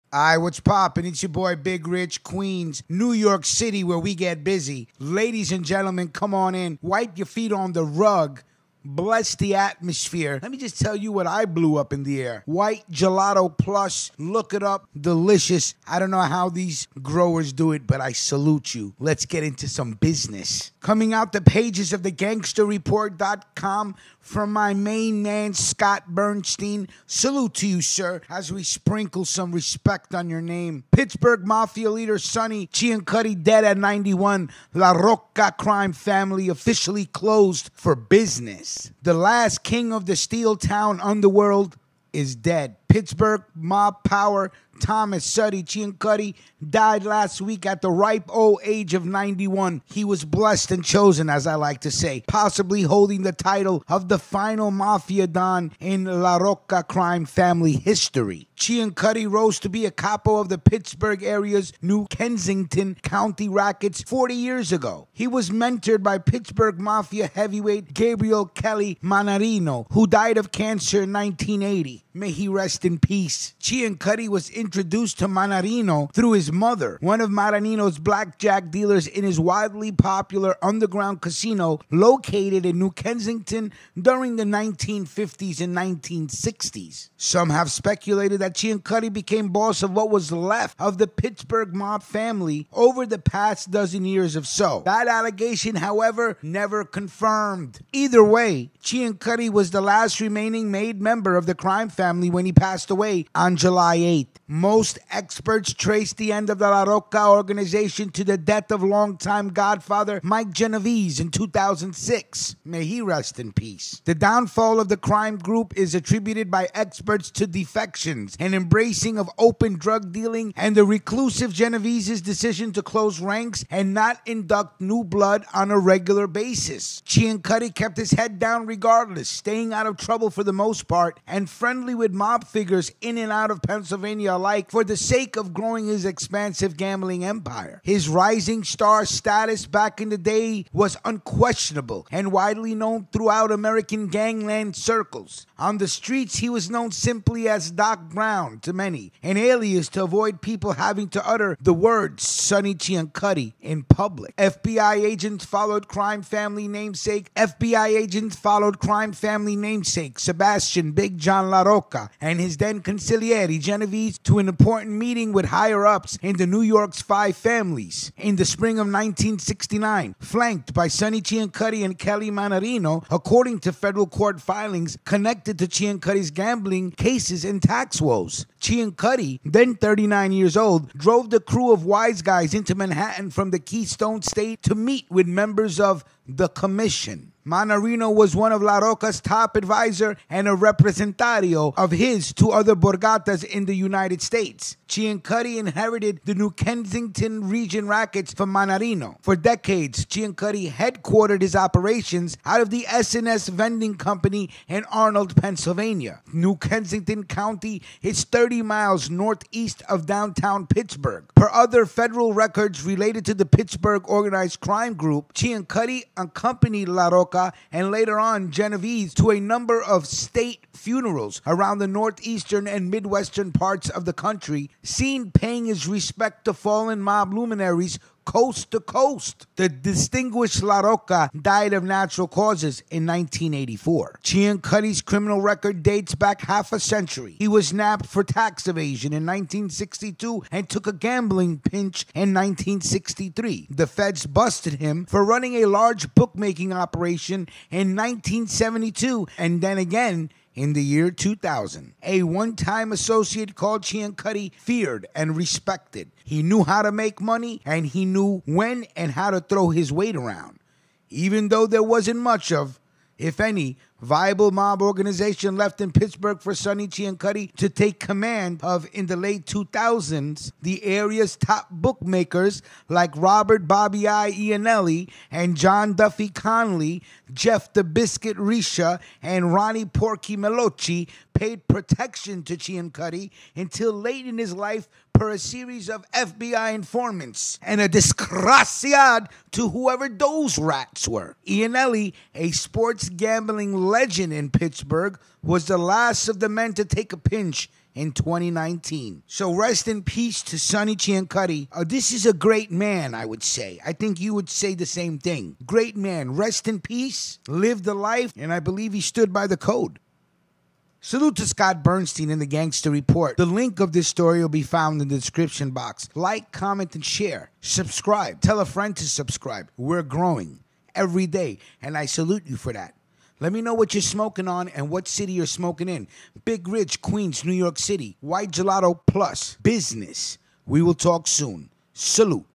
Pittsburgh Mafia Leader “Sonny” Ciancutti Dead At 91 LaRocca Crime Family Officially Closed
The last king of the Steel Town underworld is dead. Pittsburgh mob power, Thomas (Sonny) Ciancutti, died last week at the ripe old age of 91, possibly holding the title of final mafia don in LaRocca crime family history.